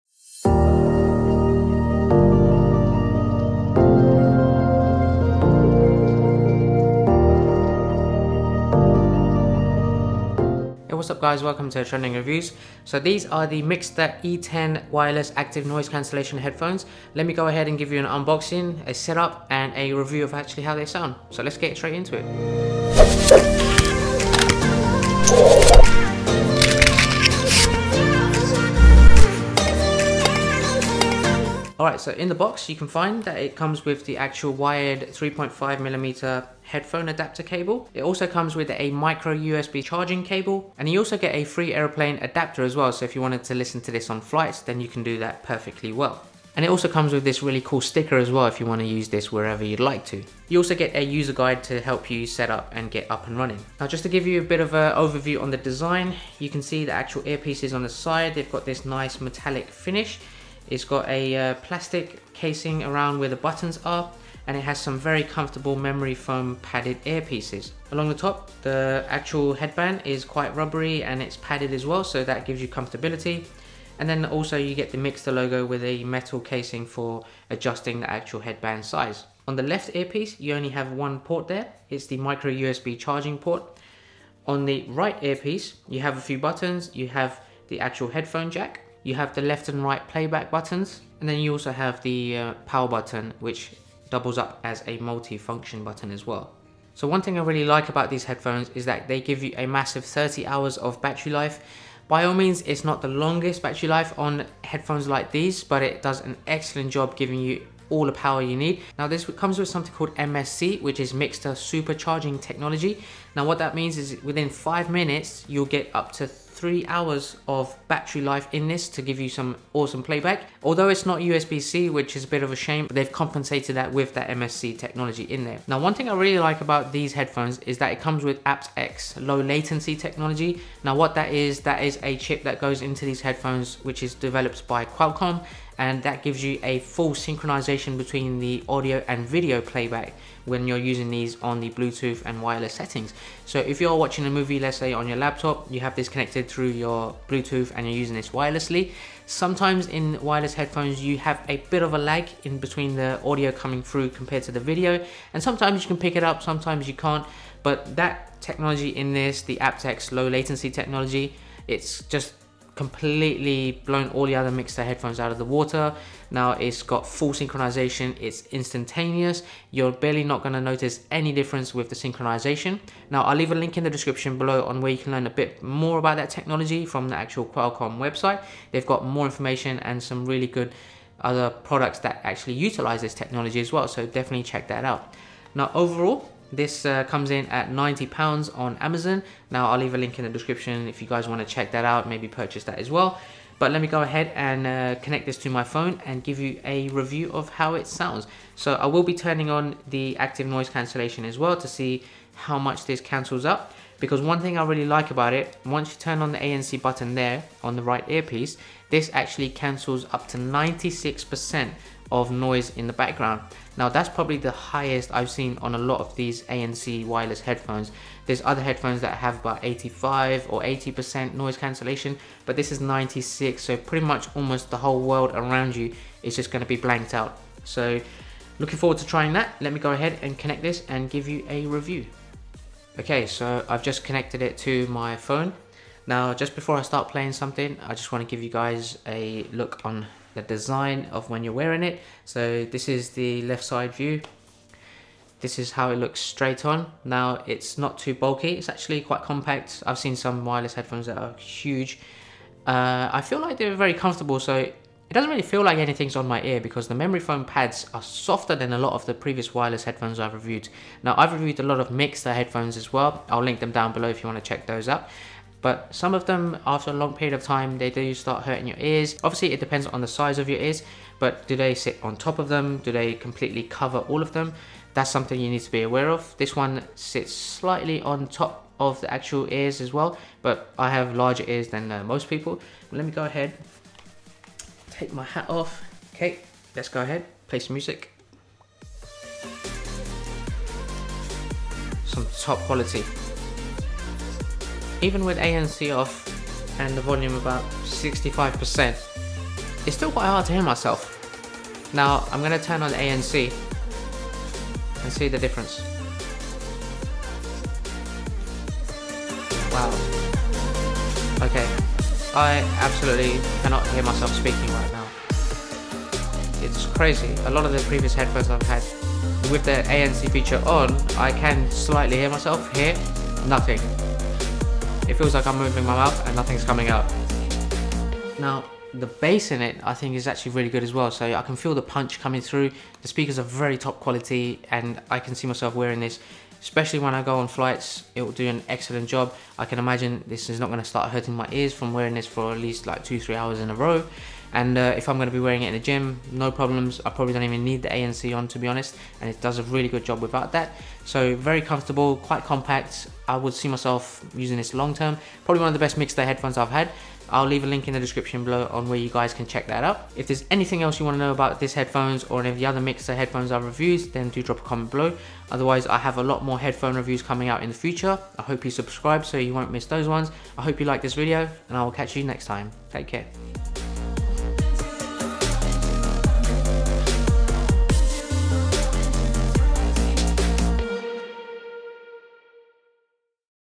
Incredible 96% Noise Cancellation! Mixcder E10 Wireless Headphones Review.
This is the 30 Hour Mixcder E10 ANC Wireless Headphones. An incredible 96% Noise Cancellation from the outside world!

Amazon UK